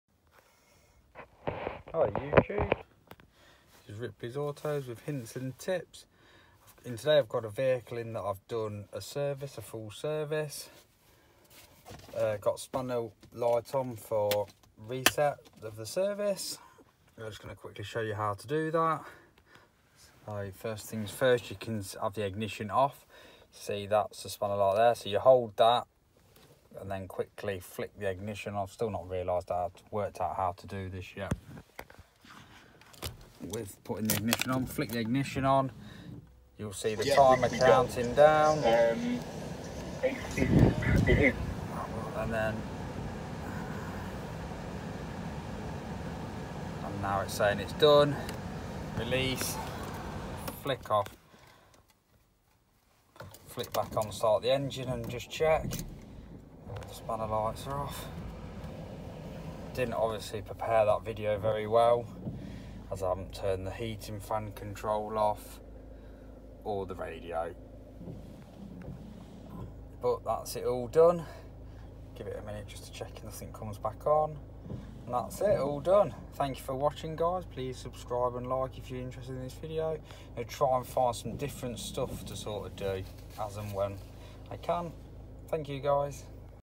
citroen berlingo /c3 service reset inpection light/ spanner light new 71plate
resetting citroen berlingo /c3 service inpection light/ spanner light